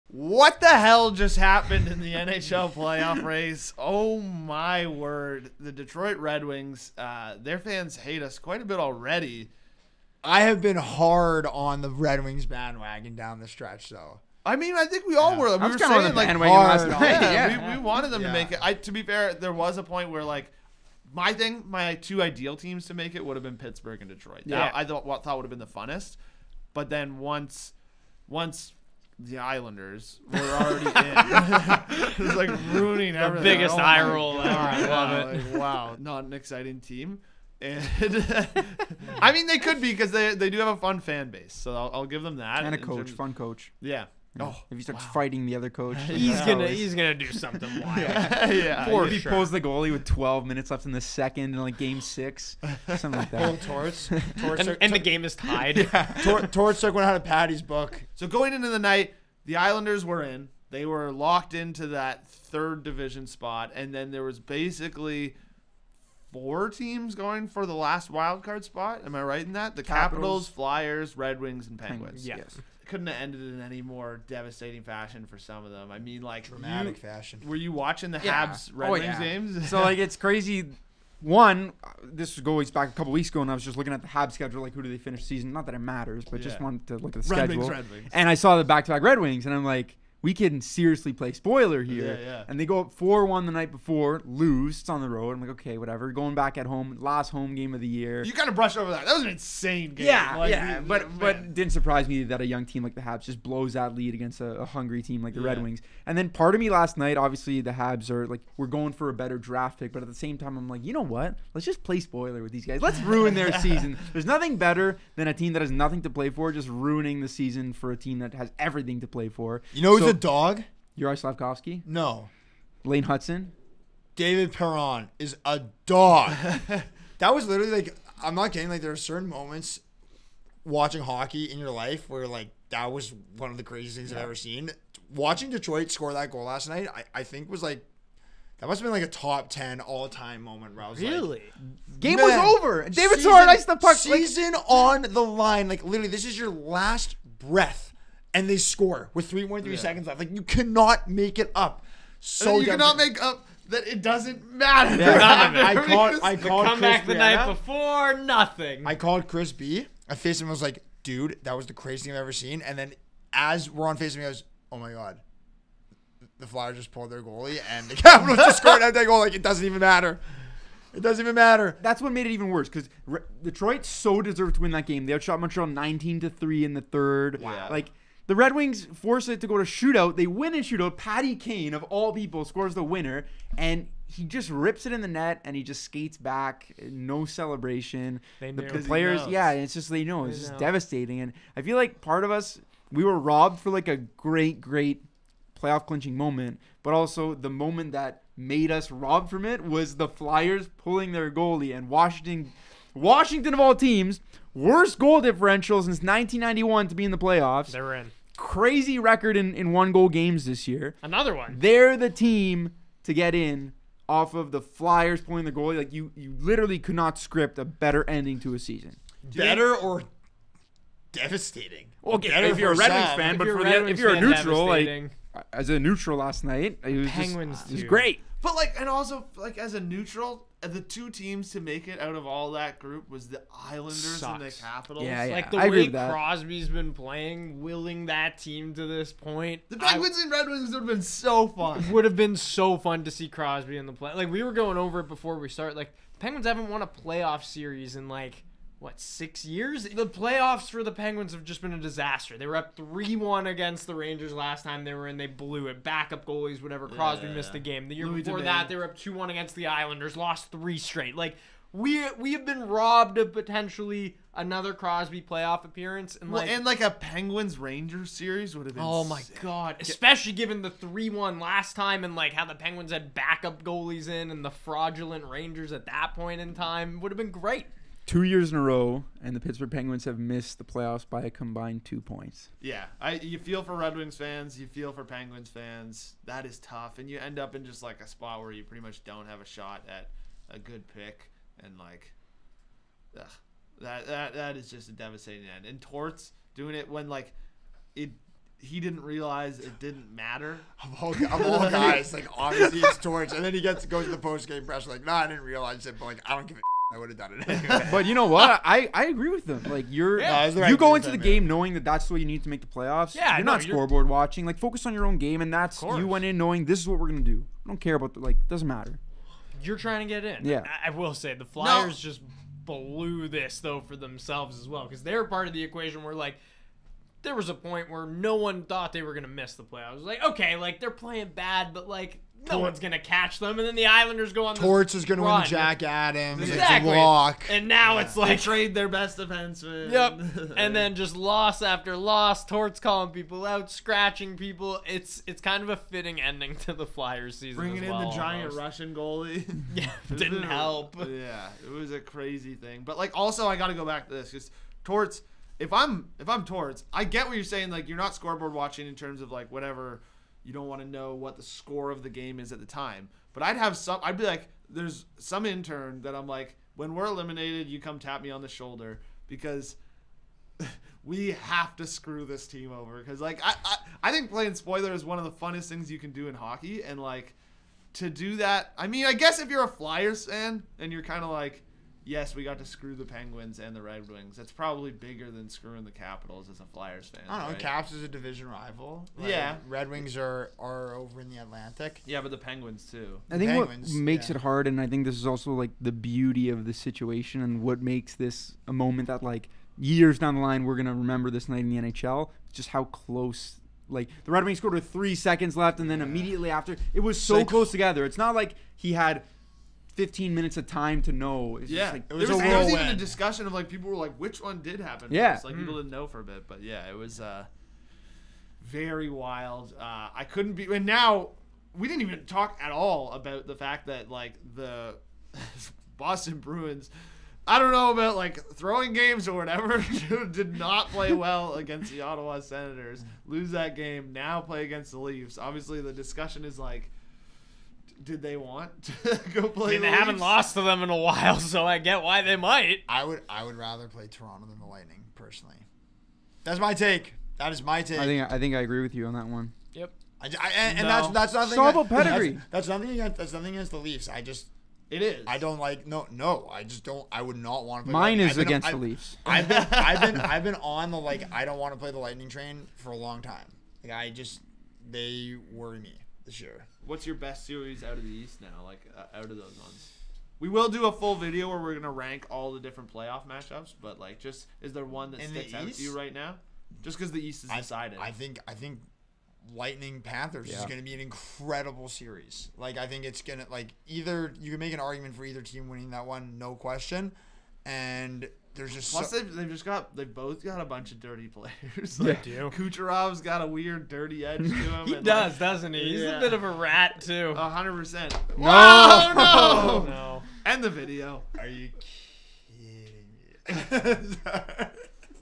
REACTION TO WILD NIGHT IN NHL PLAYOFF RACE
The Detroit Red Wings, Philadelphia Flyers and Pittsburgh Penguins were devastated by one of the oddest ways we've ever seen a team make the playoffs. Members of the BarDown Podcast got together to discuss everything that happened.